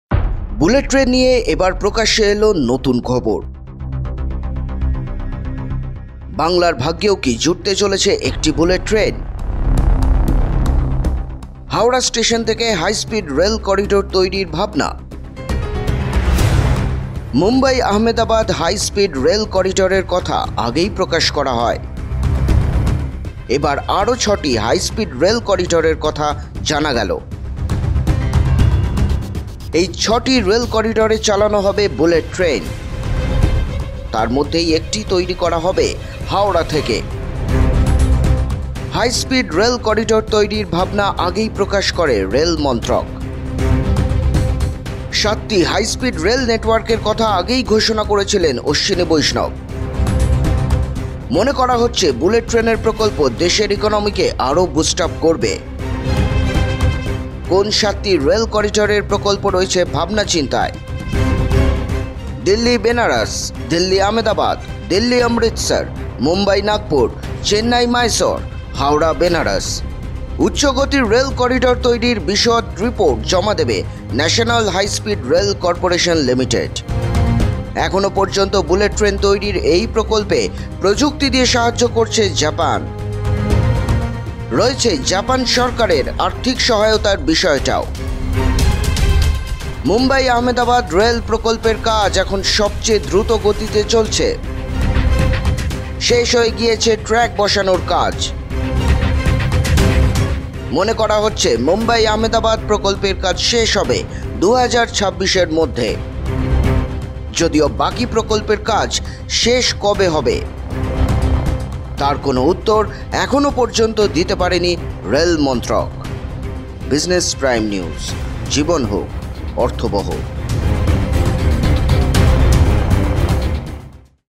হাওড়া থেকে সত্যি ছুটবে বুলেট ট্রেন? BULLET TRAIN HOWRAH | BPN
বুলেট ট্রেন নিয়ে এবার প্রকাশ্যে এলো নতুন খবর
বাংলার ভাগ্যেও কি জুটতে চলেছে একটি বুলেট ট্রেন?
হাওড়া স্টেশন থেকে হাই স্পিড রেল করিডোর তৈরির ভাবনা
মুম্বই-আহমেদাবাদ হাই স্পিড রেল করিডরের কথা আগেই প্রকাশ করা হয়
এবার আরও ৬-টি হাই স্পিড রেল করিডরের কথা জানা গেল
এই ৬-টি রেল করিডরে চালানো হবে বুলেট ট্রেন
তার মধ্যেই একটি তৈরি করা হবে হাওড়া থেকে
হাই স্পিড রেল করিডর তৈরির ভাবনা আগেই প্রকাশ করে রেল মন্ত্রক
৭-টি হাই স্পিড রেল নেটওয়ার্কের কথা আগেই ঘোষণা করেছিলেন অশ্বিনী বৈষ্ণব
মনে করা হচ্ছে, বুলেট ট্রেনের প্রকল্প দেশের ইকোনমিকে আরও এগোতে সাহায্য করবে
কোন ৭-টি রেল করিডরের প্রকল্প রয়েছে ভাবনা চিন্তায়?
দিল্লি-বেনারস, দিল্লি-আহমেদাবাদ, দিল্লি-অমৃতসর, মুম্বই-নাগপুর, চেন্নাই-মাইসোর, হাওড়া-বেনারস
উচ্চ গতির রেল করিডোর তৈরির বিশদ রিপোর্ট জমা দেবে ন্যাশনাল হাই স্পিড রেল কর্পোরেশন লিমিটেড
এখনো পর্যন্ত বুলেট ট্রেন তৈরির এই প্রকল্পে প্রযুক্তি দিয়ে সাহায্য করছে জাপান
রয়েছে জাপান সরকারের আর্থিক সহায়তার বিষয়টাও
মুম্বই-আহমেদাবাদ রেল প্রকল্পের কাজ এখন সবচেয়ে দ্রুত গতিতে চলছে
শেষ হয়ে গিয়েছে ট্র্যাক বসানোর কাজ
মনে করা হচ্ছে, মুম্বই-আহমেদাবাদ প্রকল্পের কাজ শেষ হবে ২০২৬-এর মধ্যে
যদিও বাকি প্রকল্পের কাজ শেষ কবে হবে?
তার কোন উত্তর এখনো পর্যন্ত দিতে পারেনি রেল মন্ত্রক